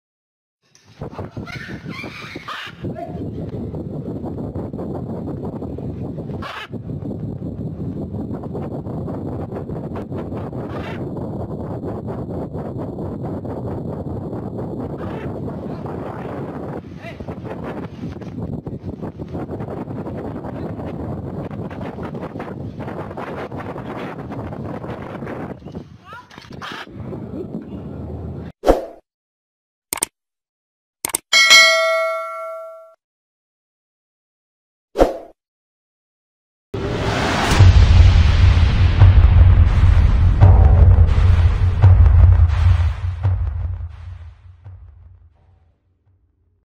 ببغاء يصور بث حي وهو يحلق بعد ان سرق احد الهواتف وطار
متداول.. فيديو يظهر قيام ببغاء بسرقة هاتف محمول والطيران به لمسافة قبل العودة سالما إلى الأرض 🤣
ببغاء يسرق موبايل أثناء تشغيل كاميرته ويصور الحي
ببغاء يسرق موبايل أثناء تشغيل كاميرته ويصور الحي " فيديو كامل ". Parrot Thief